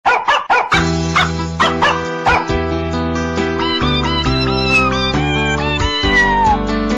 Claws vs. Jaws
If a dog were to jump on a cat and playfully bite her face, the cat's most probable reaction would be a defensive one. The cat may hiss, scratch, or swat at the dog to establish boundaries and protect herself. She might also retreat to a safe location or vocalize her displeasure. It's important to supervise interactions and ensure the safety and well-being of both pets.